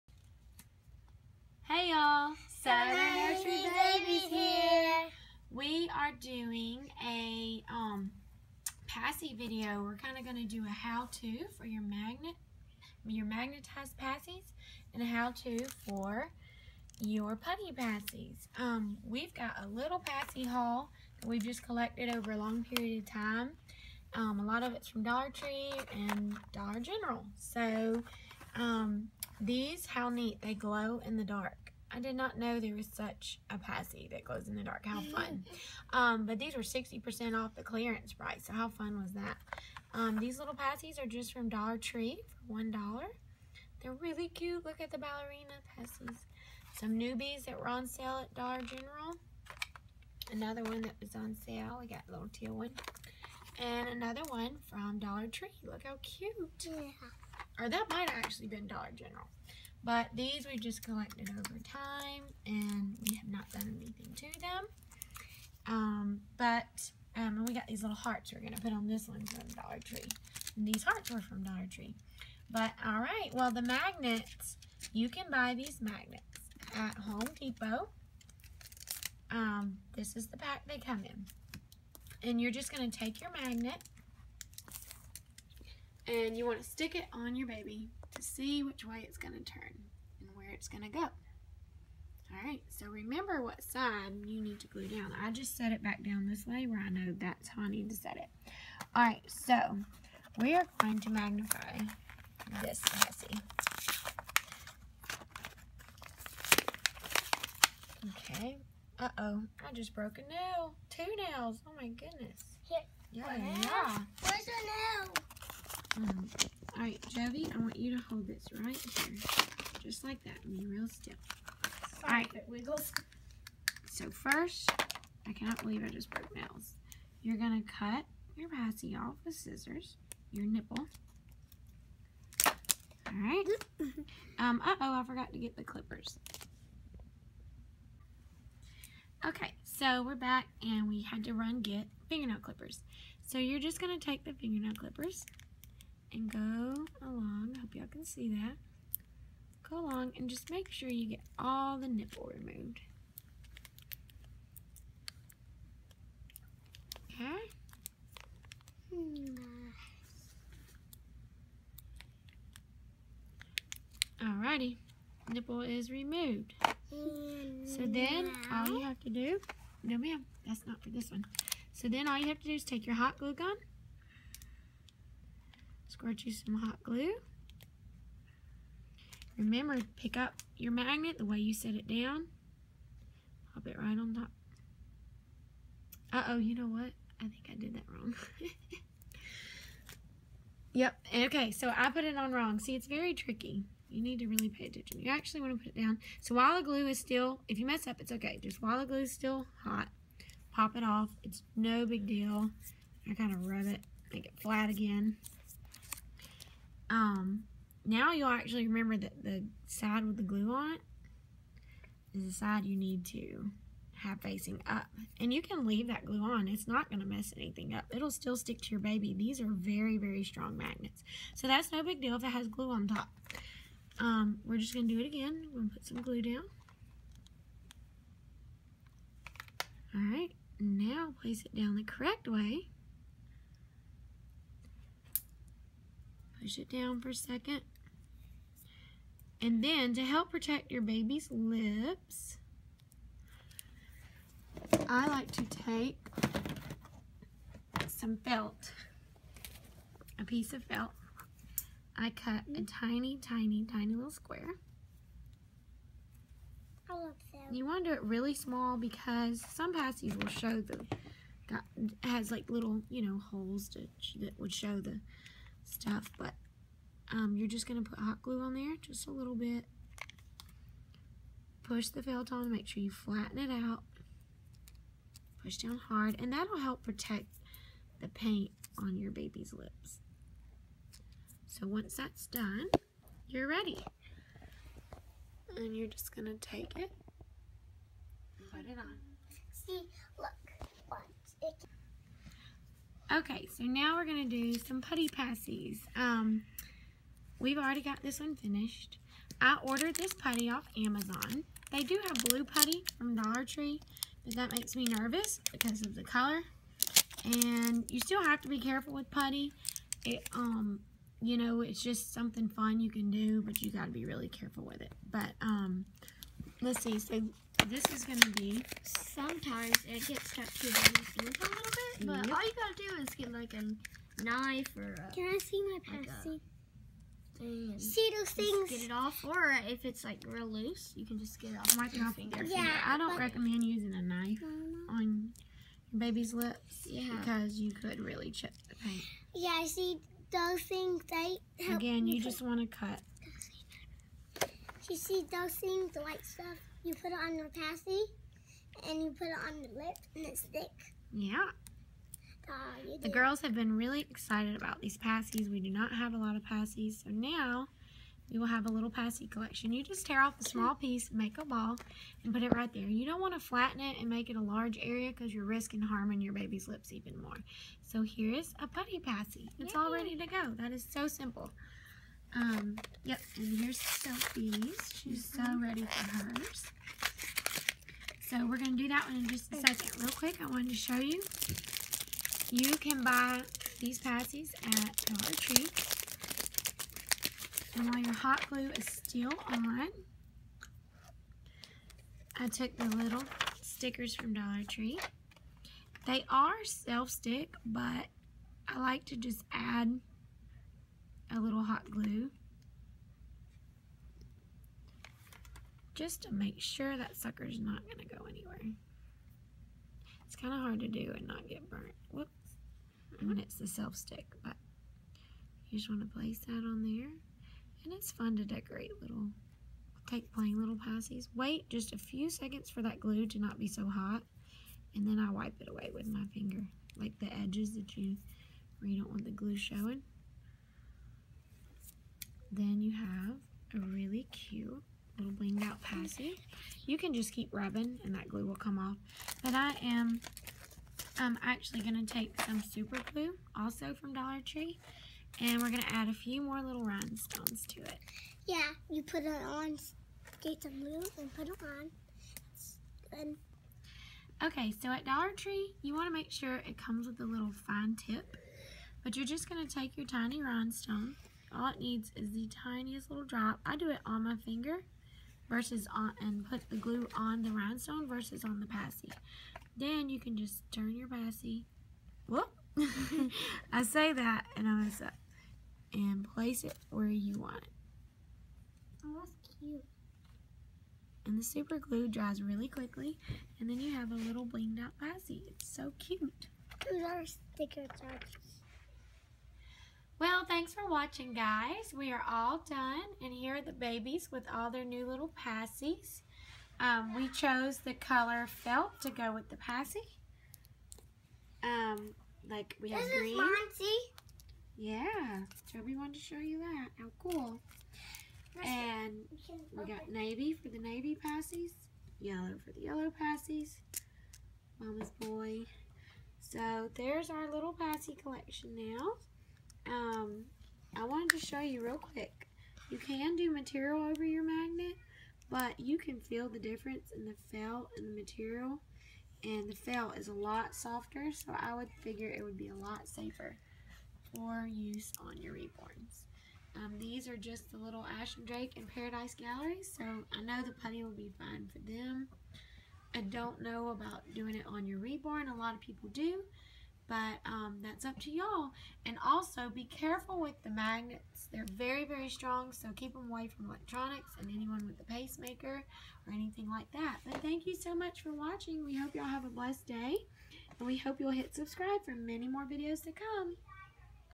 How to make Magnetic and Putty Pacifiers for reborn dolls!!!
This is a how to on making a magnetized pacifier and a putty pacifier for your reborn doll. Plus our pacifier haul!!